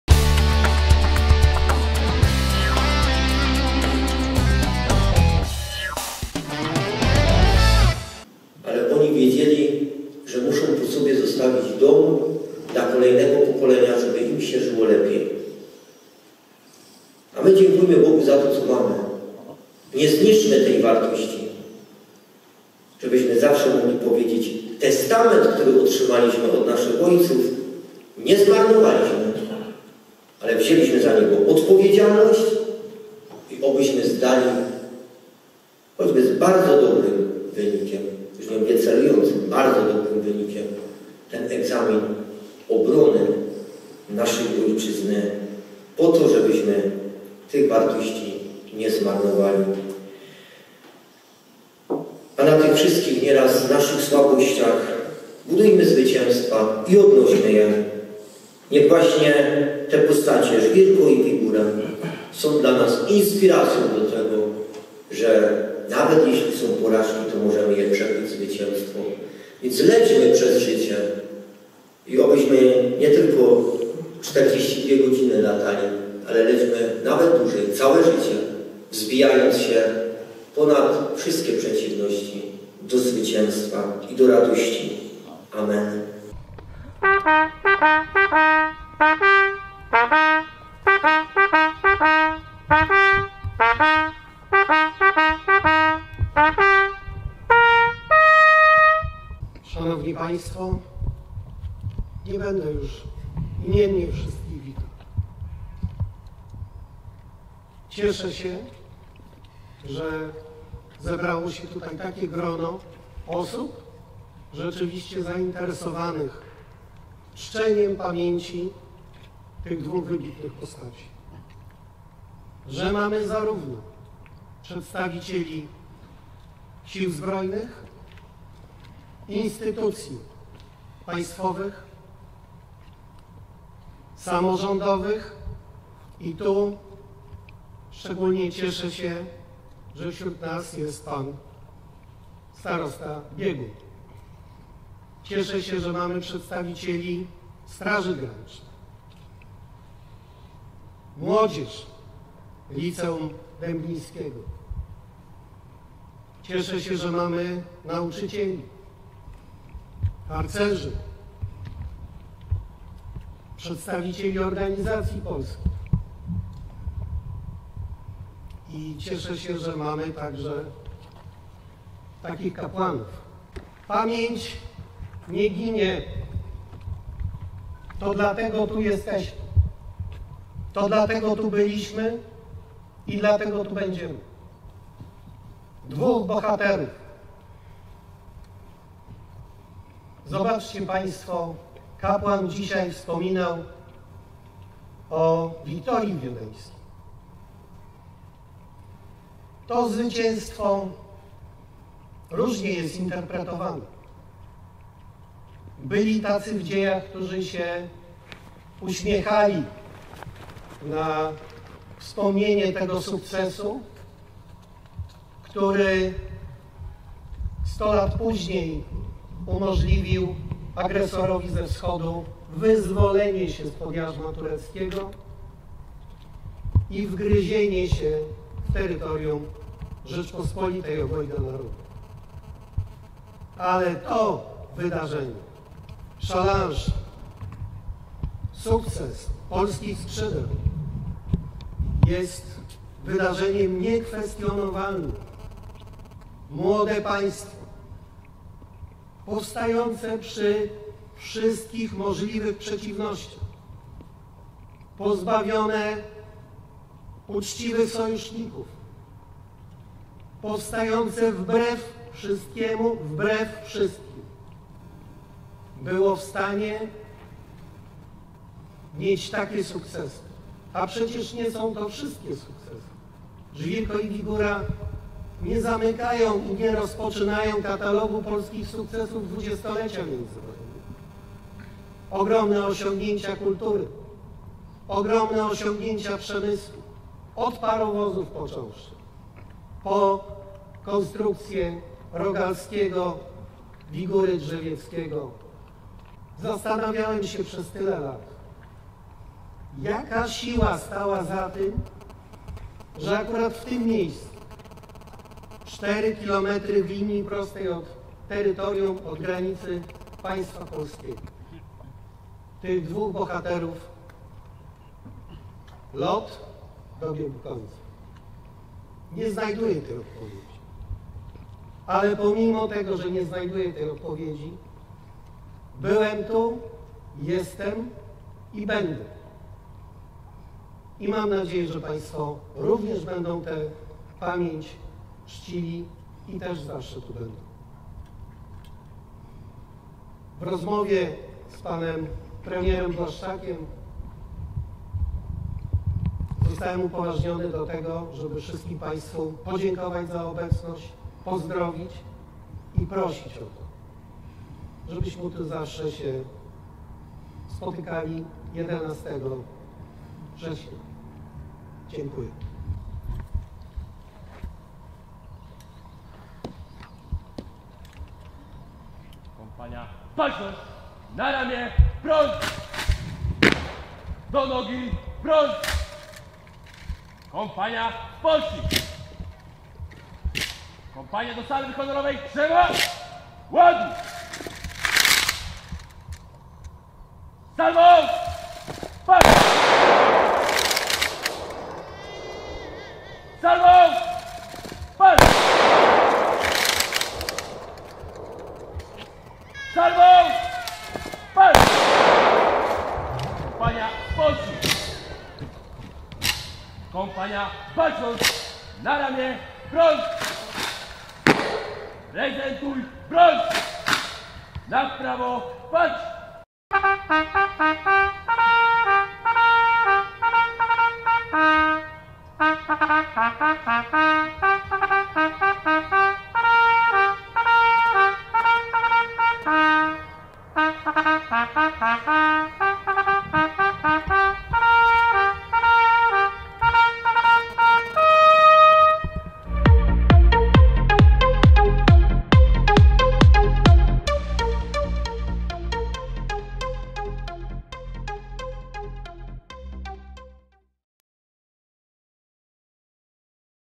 Uroczystość wspomnieniowa z okazji 91. Rocznicy tragicznej śmierci Żwirki i Wigury
W poniedziałek (11.09) w Cierlicku na Żwirkowisku odbyła się Uroczystość wspomnieniowa z okazji 91. Rocznicy tragicznej śmierci Żwirki i Wigury.
Uroczyste obchody rocznicy, organizowane są od dziesiatek lat z inicjatywy miejscowego Koła Polskiego Związku Kulturalno-Oświatowego w Republice Czeskiej, jako społecznego opiekuna miejsca tragedii wybitnych lotników, mają na celu godne uczczenie ich pamięci a zarazem podkreślenie tradycji i osiągnięć polskiego lotnictwa.